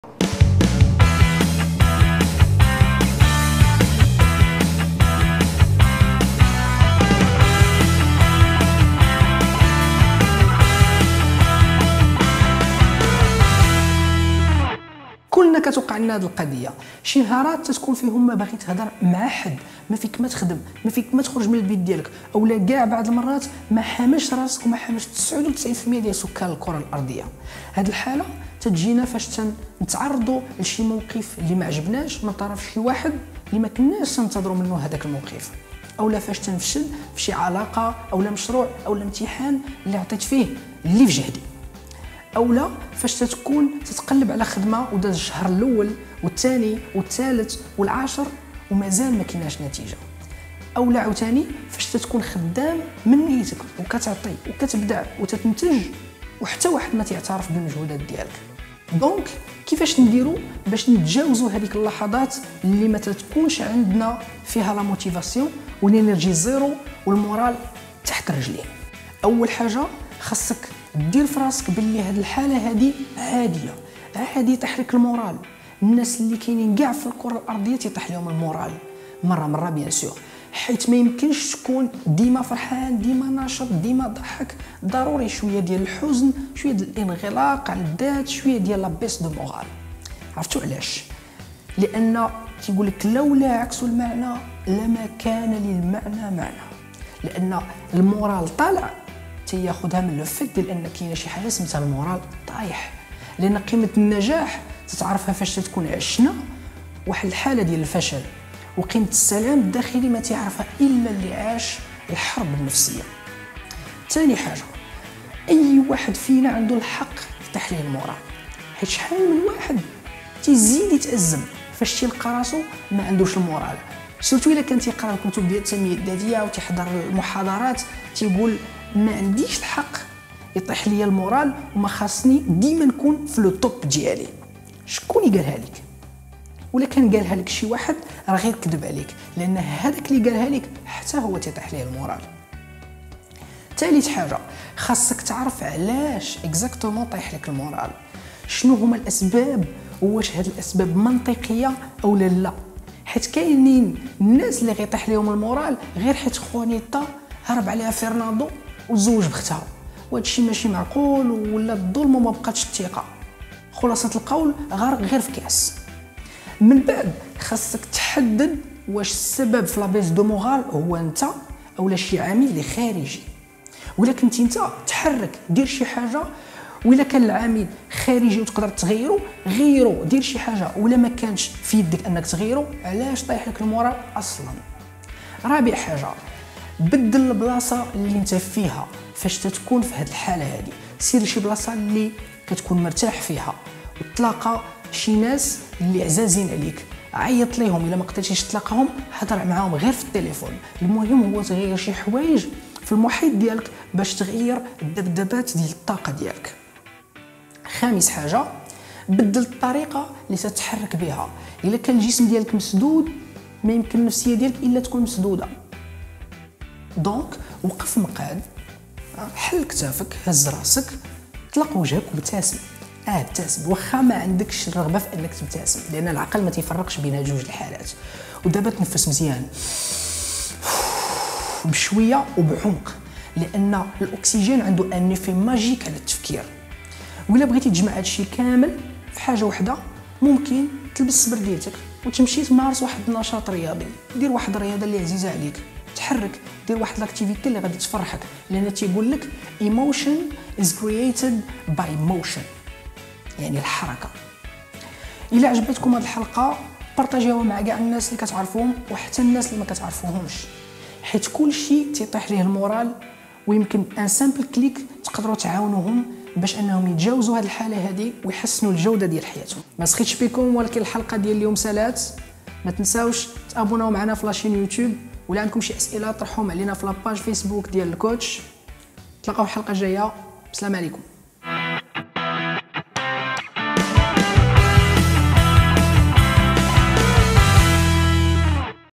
L'Coach Ep:6 -Les 5 Etapes pour dépasser la baisse de moral لكوتش الحلقة 6
في هذه الحلقة سنتطرق إلى الحديث عن الأسباب التي تحبطنا وتحبط معنوياتنا، كما سنطرح مجموعة من الإشياء التي ستساعدنا على رفع من معنوياتنا والحفاظ عليها.

لكوتش هو برنامج كي تطرق لمجموعة من المواضيع لي كتهم الحياة الشخصية والمهنية ديال المغاربة.

متنساوش تأبوناو معانا في القناة يوتوب ديالنا :)